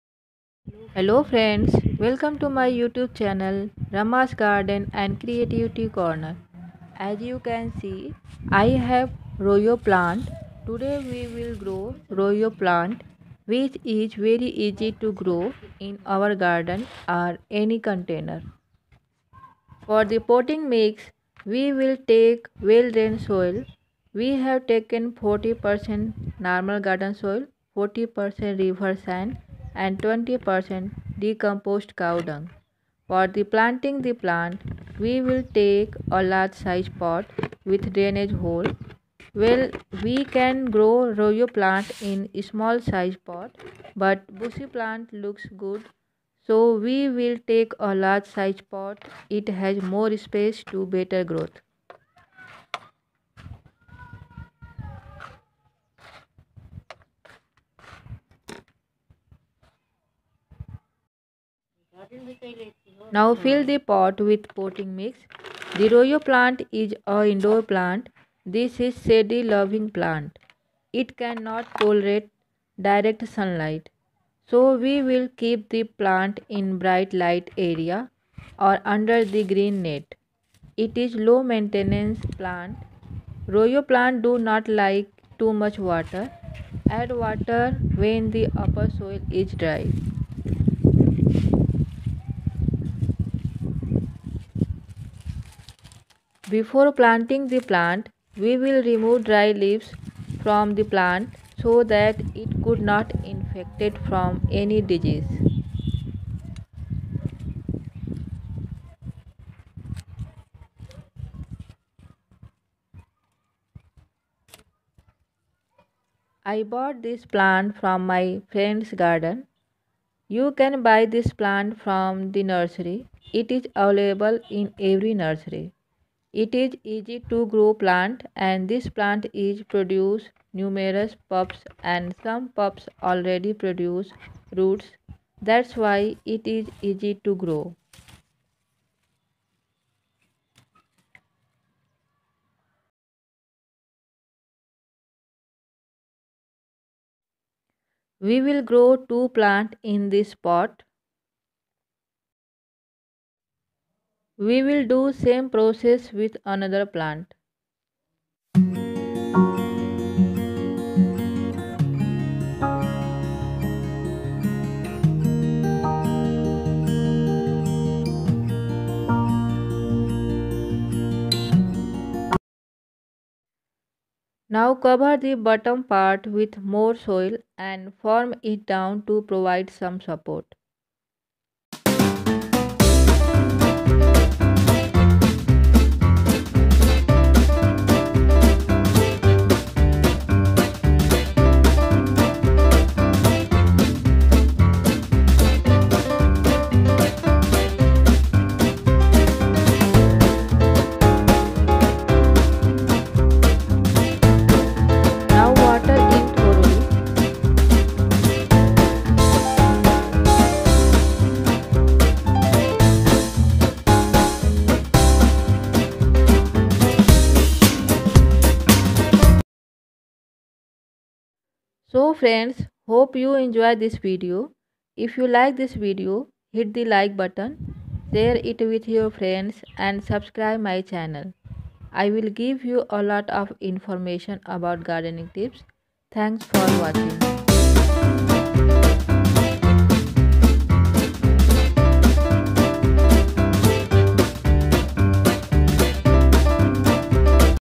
How To Grow Rhoeo Plant
Rhoeo plant is a very attractive plant. It is indoor plant and this plant is very easy to grow in our garden.